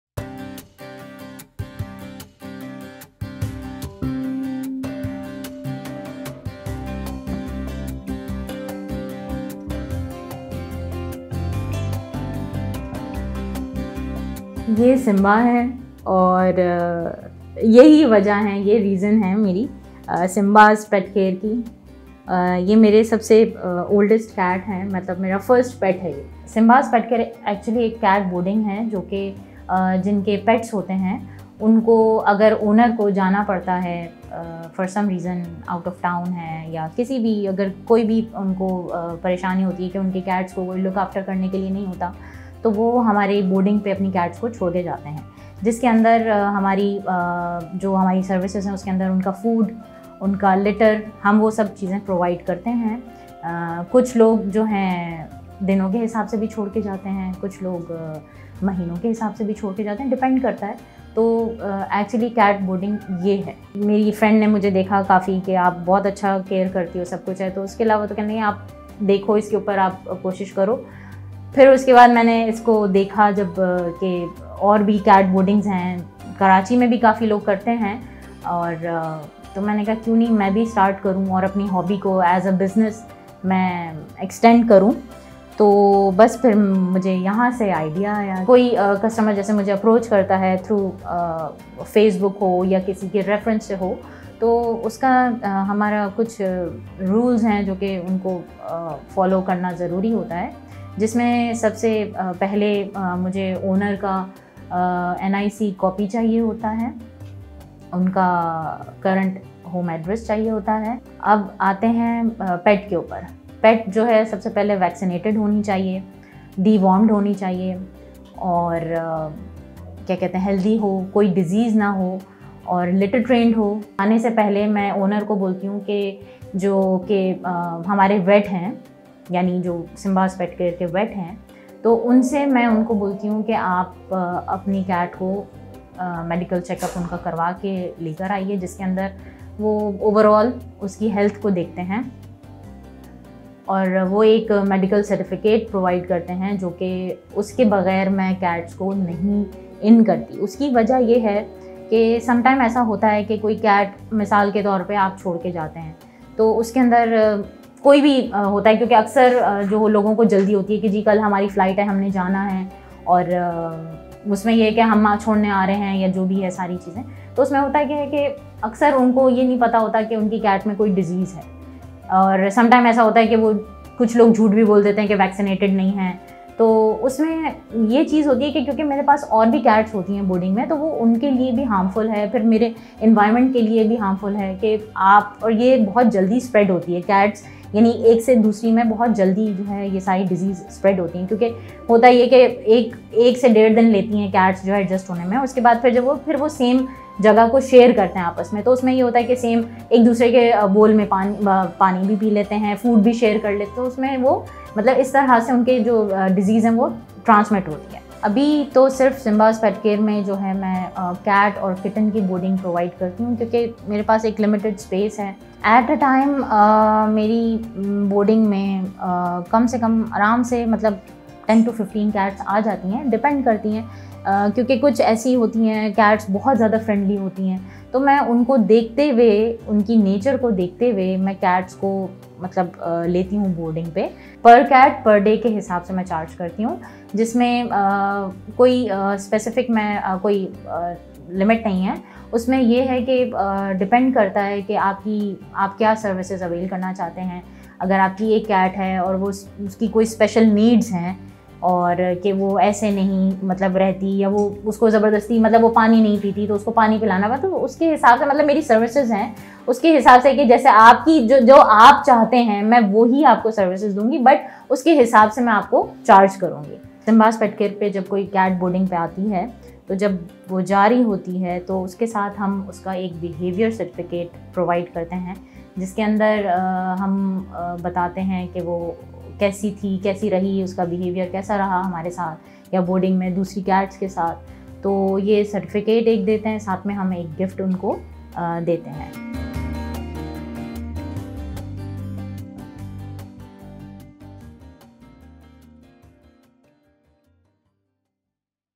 Shaista baji's boarding for billis
Shaista Rana runs Simba's pet care, a venture that offers boarding services for cats. It is a sanctuary that will feed your pet and take care of it in your absence. Shaista says her work doesn't weigh her down because she loves what she does. Watch the video to meet all her feline friends.